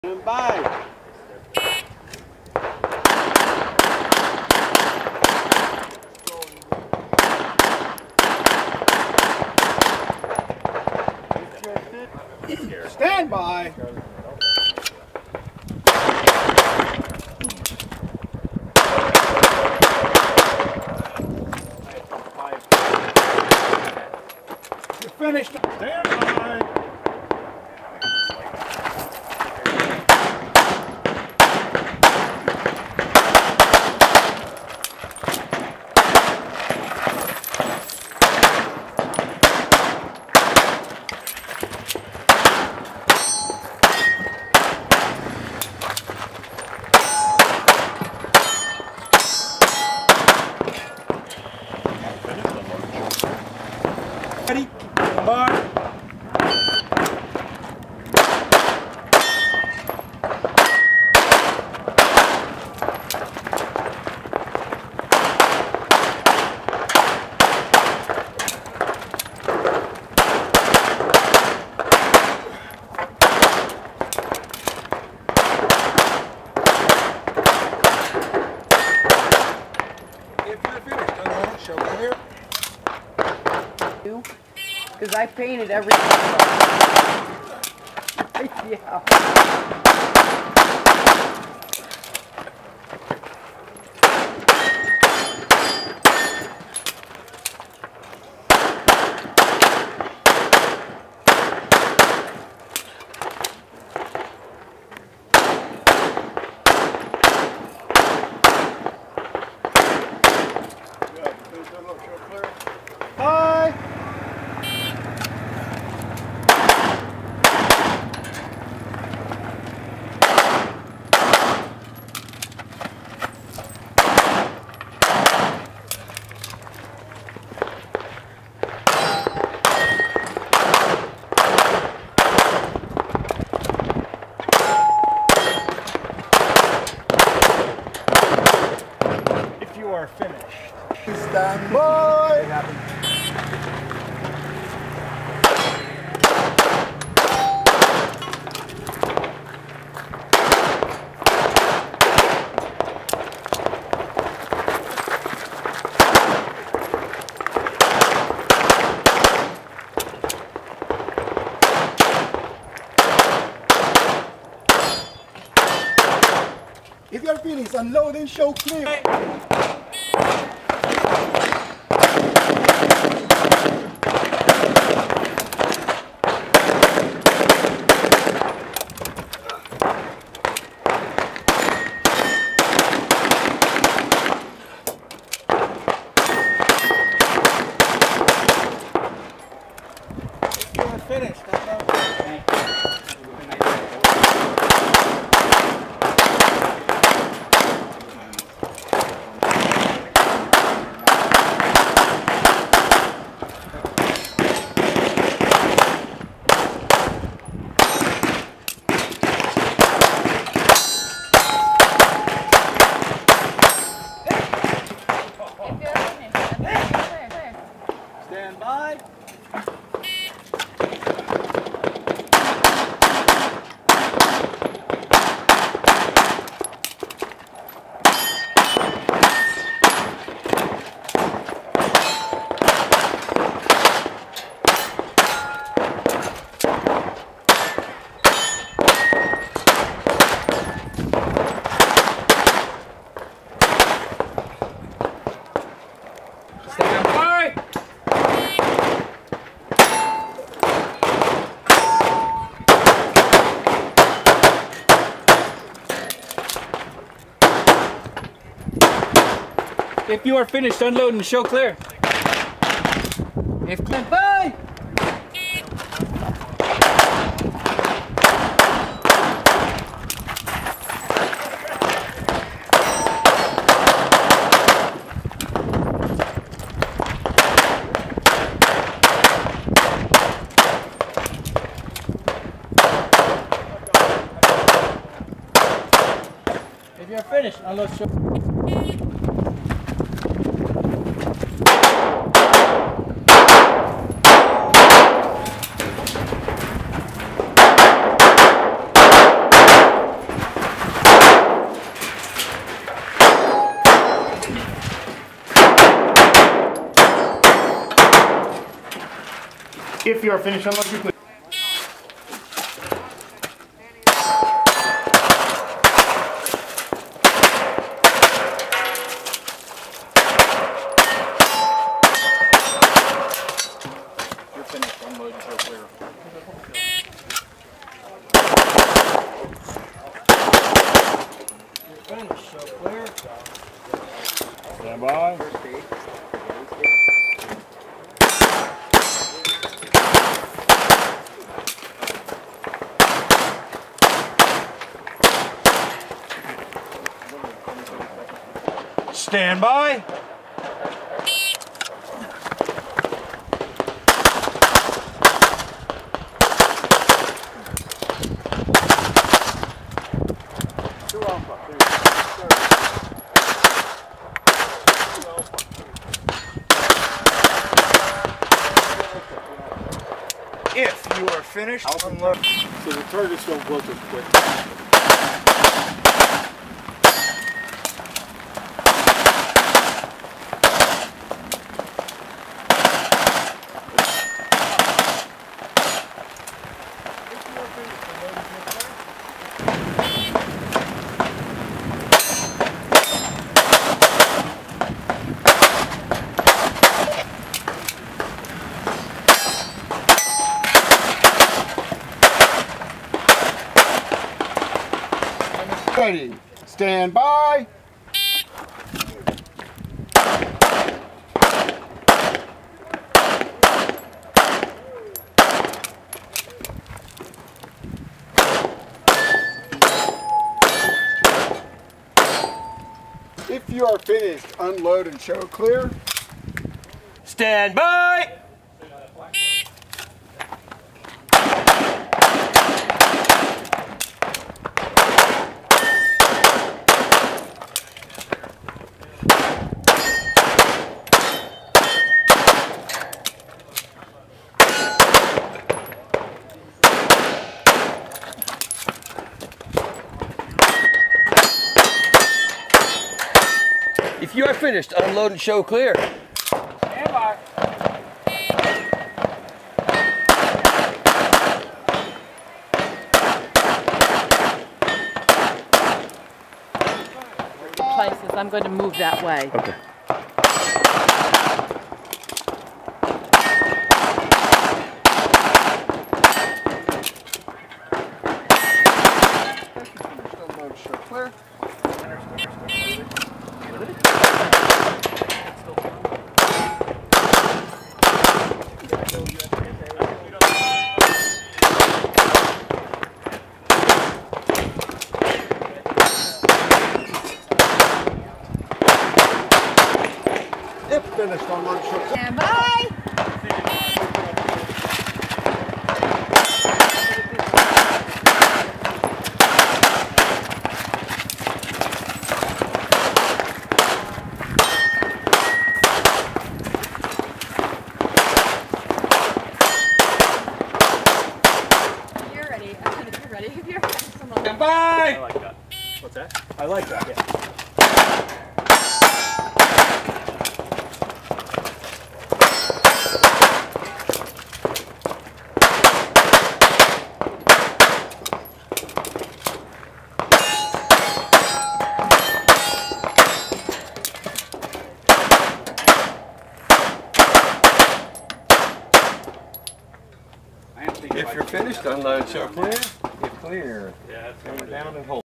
Nick Yanutola - 2014 USPSA National Championship top 16
All but one stage of my run at the 2014 USPSA National Championship (stage 19 is missing).  I was fortunate enough to meet my goal of reaching the recognized top 16 by finishing 13th overall in Production.

Thanks to SharpShooters Gun Club in Greenville SC for all the support.